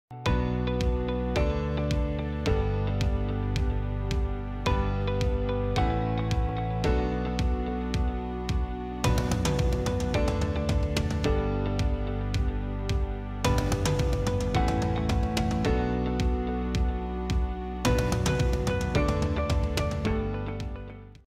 30 Sec News Bulletin | 09 June 2021 | Latest Travel News
TravClan 30 Sec News Bulletin

1. Centre issues SOPs for states for global travel

2. Hawaii Making Plans For Full Reopening, Announcing Next Steps

3. IndiGo expects domestic travel recovery by Oct-Dec

TravClan Technology India Private Limited